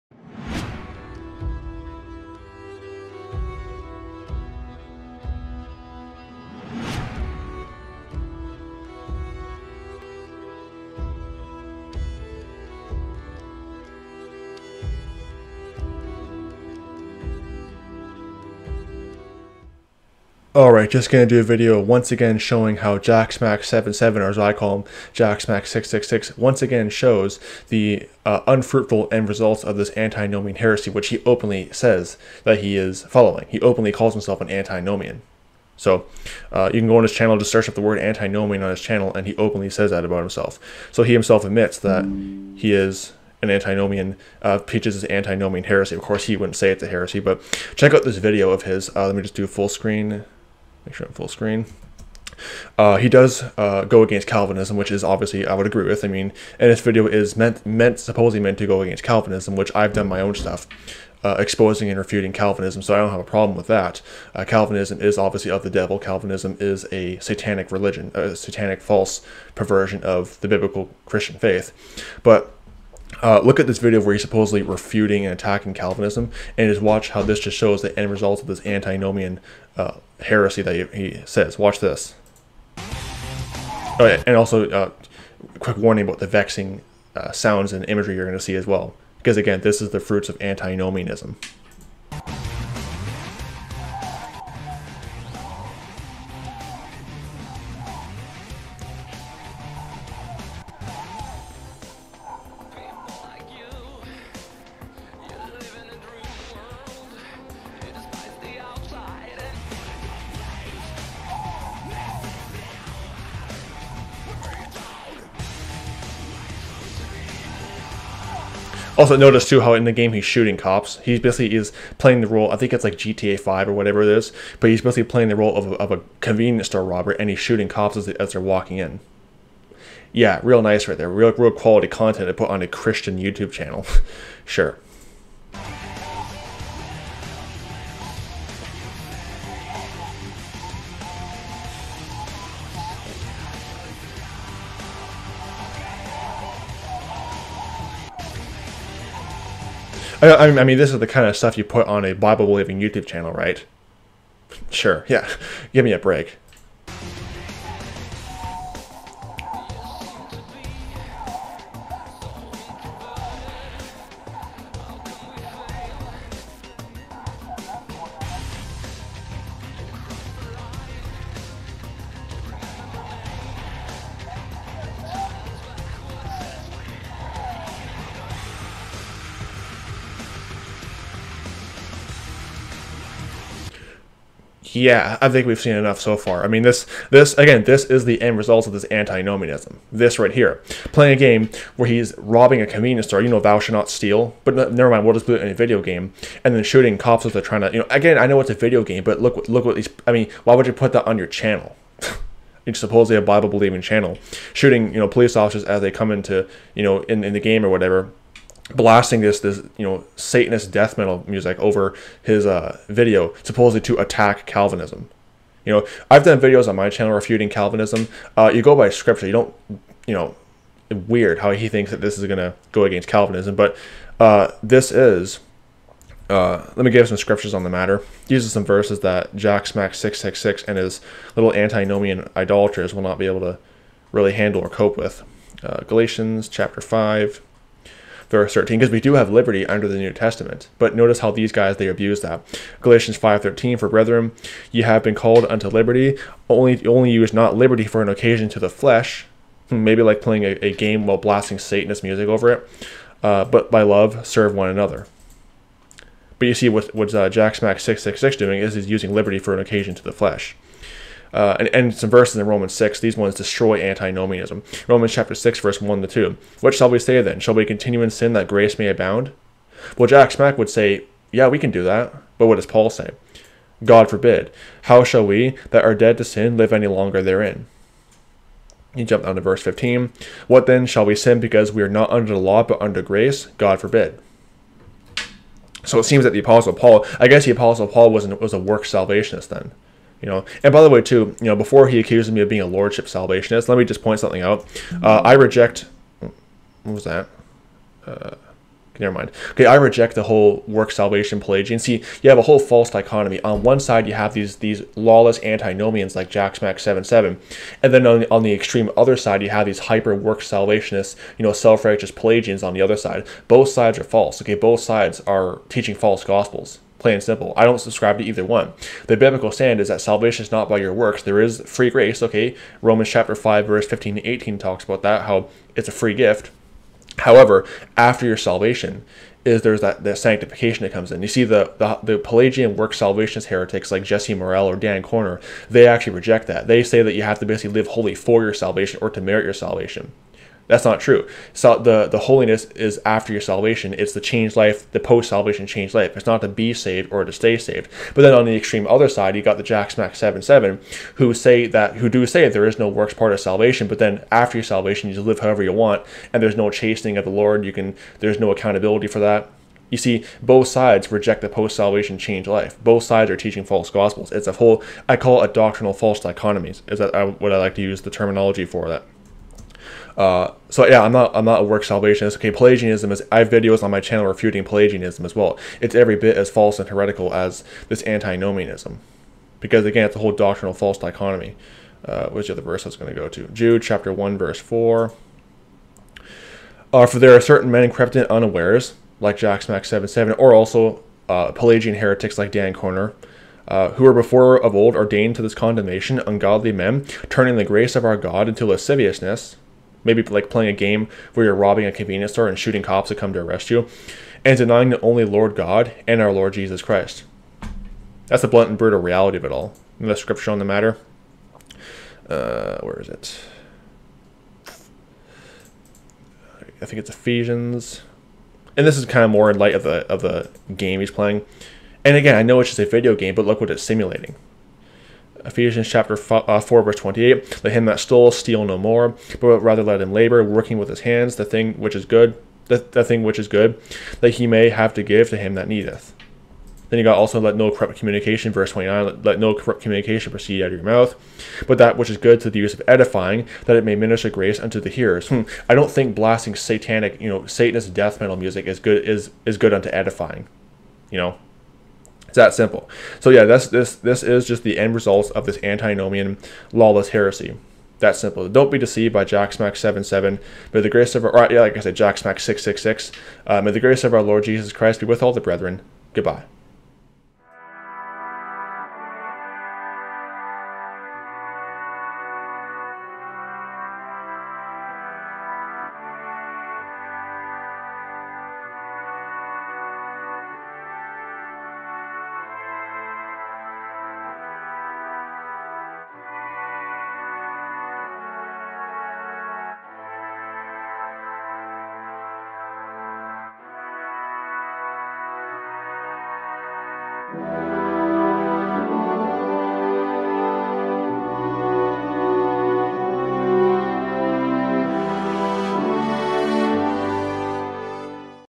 Jacksmack666 & The End Results Of Antinomian Heresy
Jacksmack77 (Jacksmack666) continues to show the unfruitful wicked end results of antinomian heresy. The antinomian heresy of Jacksmack666 is demolished in Galatians 5:13, Romans 6:1-2, Romans 6:15, Jude 1:4, and 1 Peter 2:16.